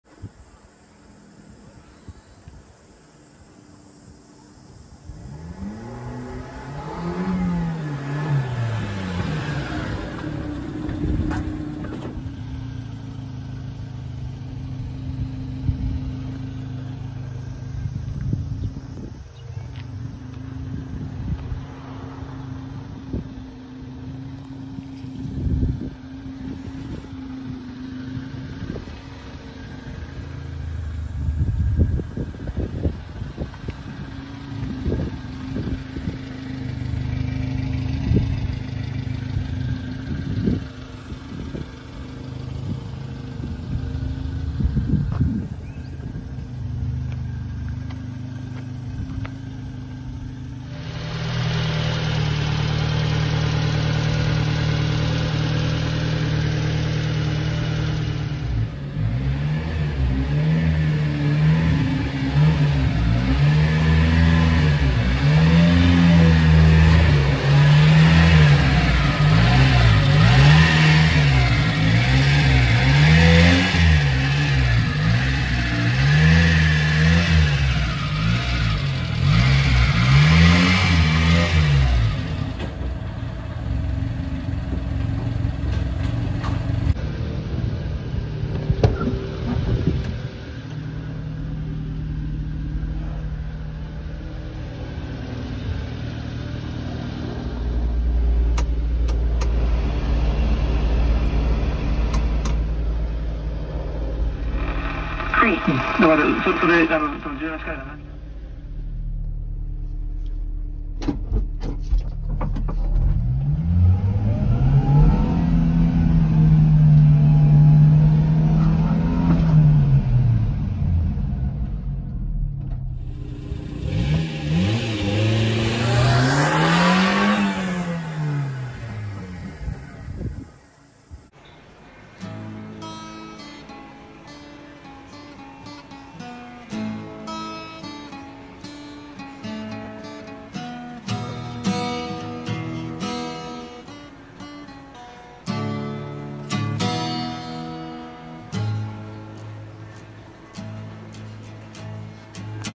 Sold out 1993 Subaru sambar truck KS4-165305
1993 SUBARU SAMBAR TRUCK
Very clean vehicle
4WD
Genuine low mileage 83900km
5speed manual and extra low (EL)
New tires